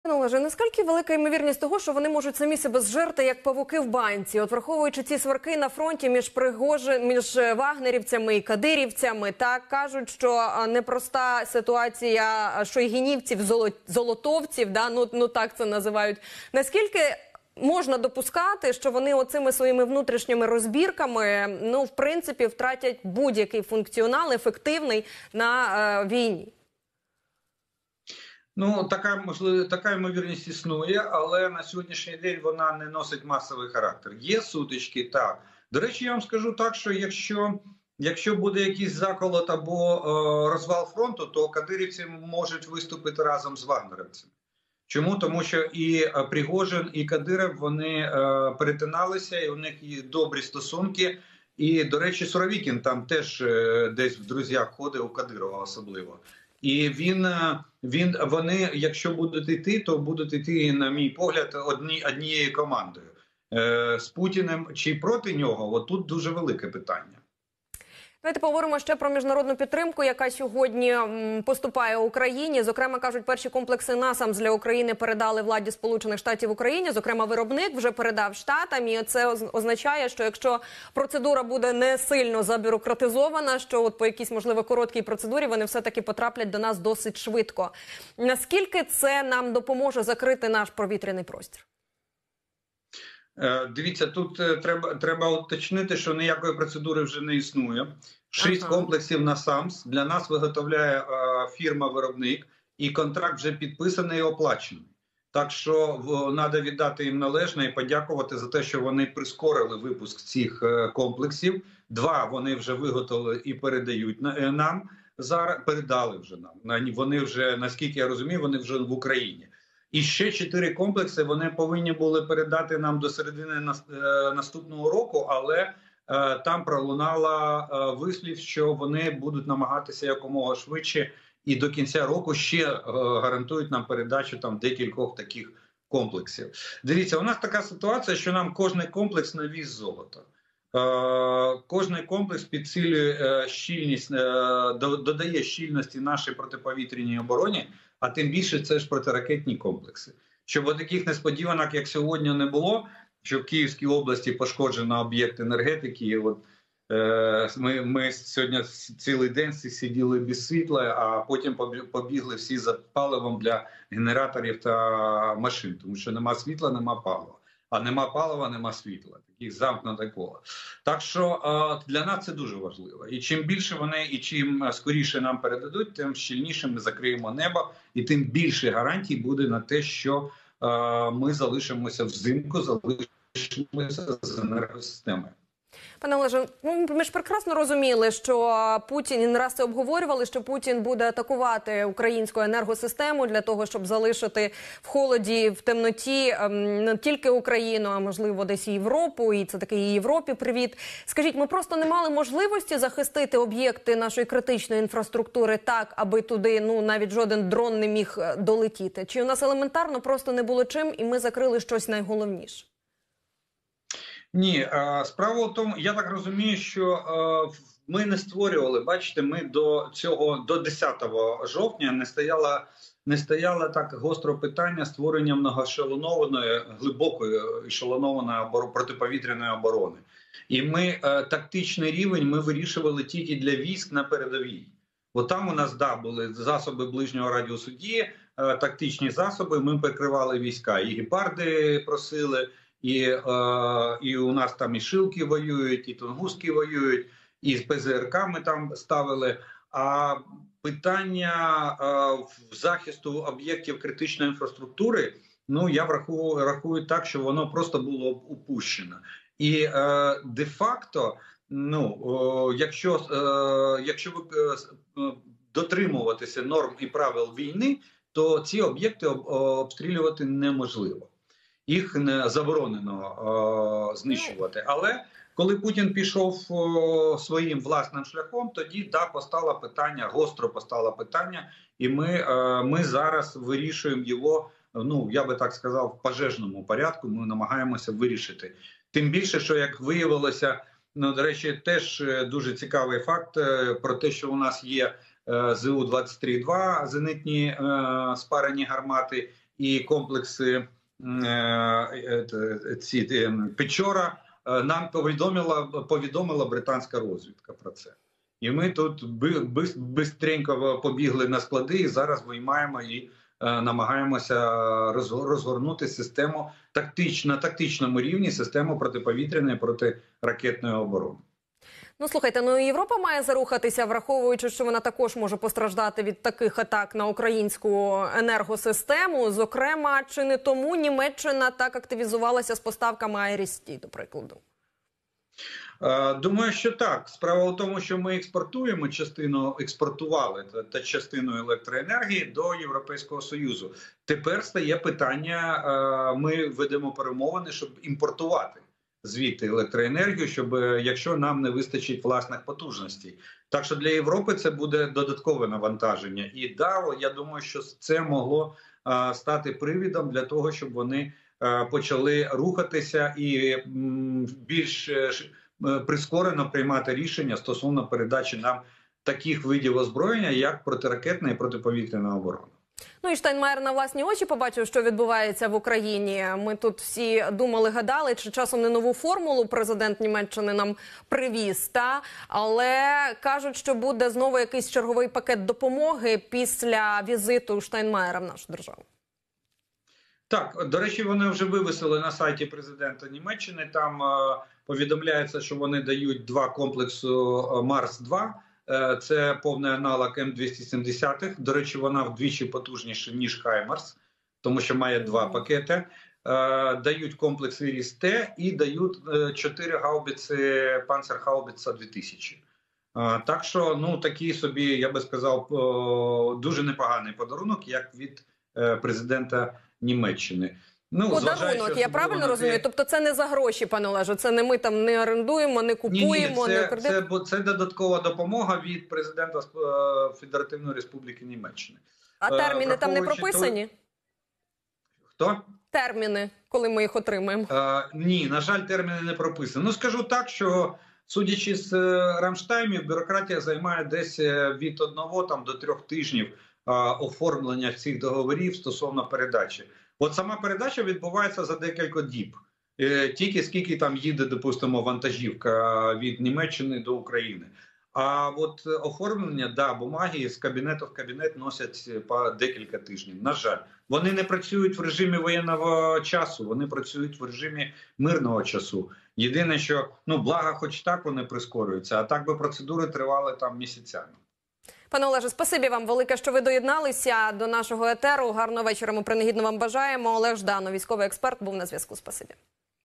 ОБСТРІЛИ : Україна швидко розгортає тактичні системи ППО // ЖДАНОВ
В ефірі: @OlegZhdanov